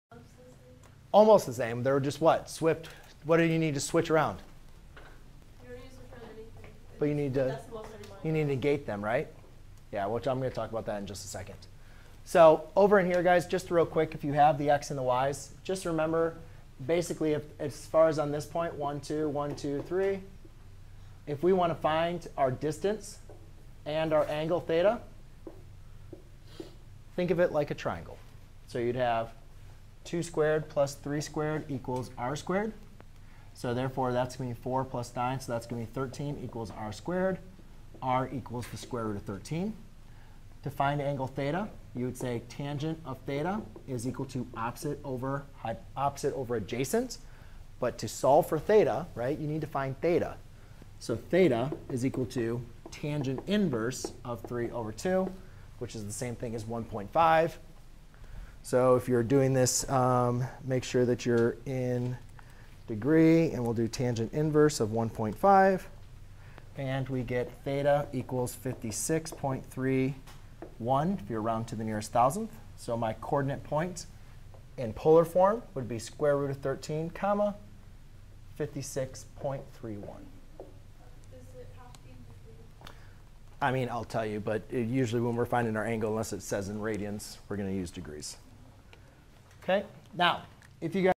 Convert a rectangular point to polar form
Learn how to convert points between rectangular and polar.  Rectangular points measure the horizontal  and vertical distance away from the origin whereas polar coordinates measure the direct distance and angle in standard position from the origin.

To convert from rectangular points to polar points, given a rectangular point (x, y), then r = sqrt(x^2 + y^2) and theta = arctan (y/x). To convert from polar points to rectangular points, given polar points (r, theta), then x = rcos(theta) and y = rsin(theta).